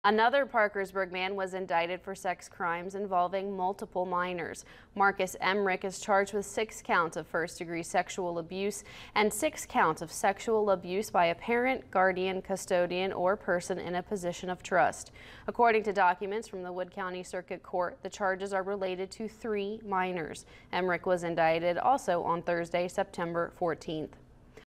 Parkersburg man charged with 12 sex crimes involving minors
Marcus J. Emrick, 33, is charged with six counts of first-degree sexual abuse and six counts of sexual abuse by a parent, guardian, custodian...